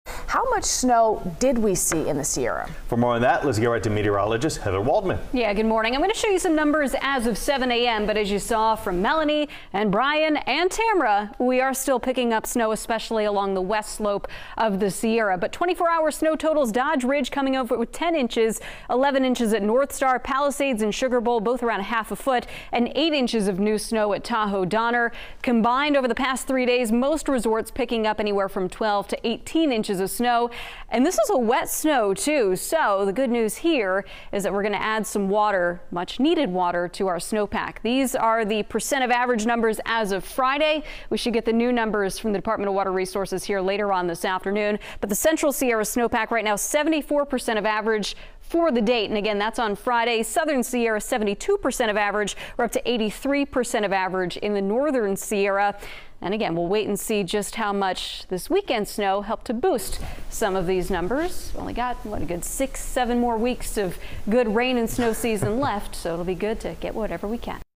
Here's a look at California's snowpack and recent snow totals
Heather Waldman breaks down three-day snow totals at Sierra resorts and looks at where things stand with California's snowpack, which is key source for the state's water supply.